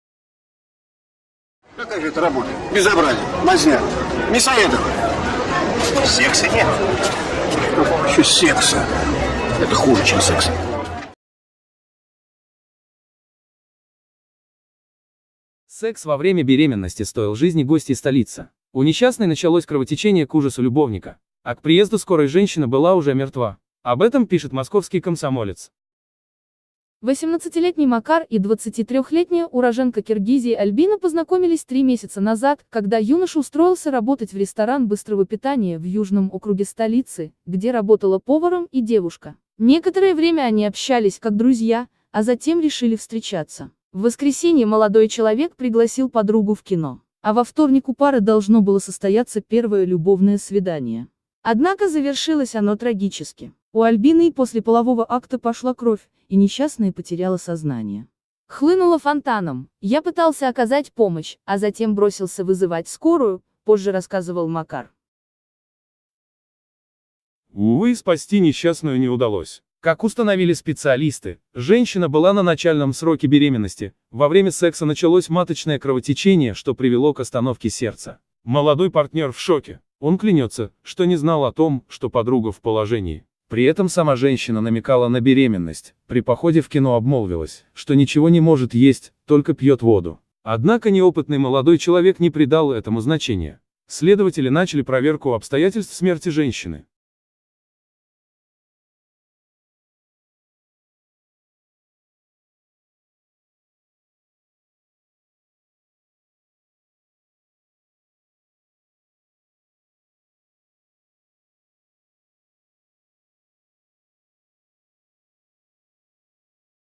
В Москве беременная повариха умерла во время секса
Секс во время беременности стоил жизни гостье столице. У несчастной началось кровотечение к ужасу любовника. А к приезду «скорой» женщина была уже мертва.